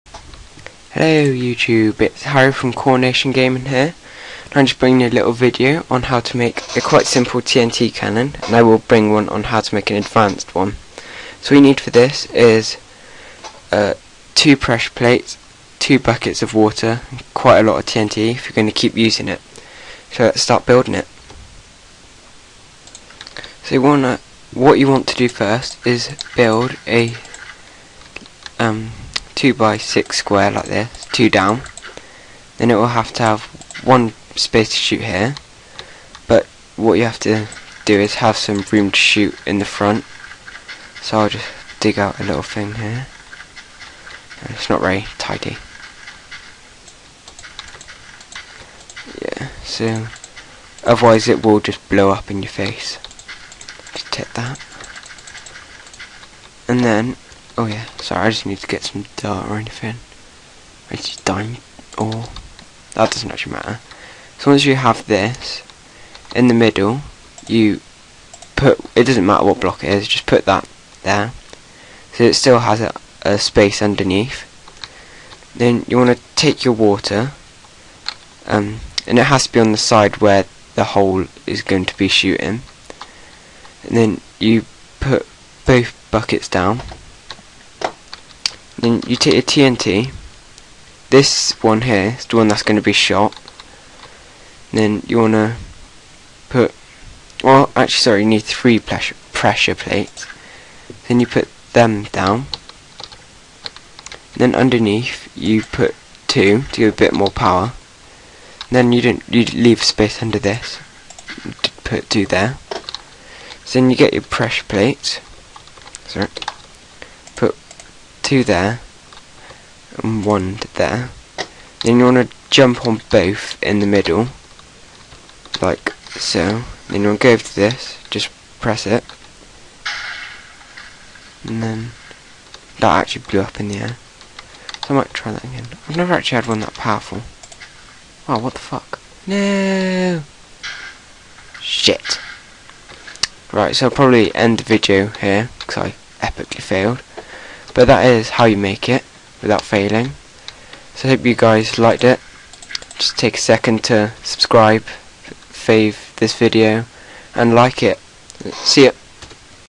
Harrys Helpful Hints-Simple TNT Cannon
A very simple but effective TNT cannon! You need 3 pressure plates not 2!!!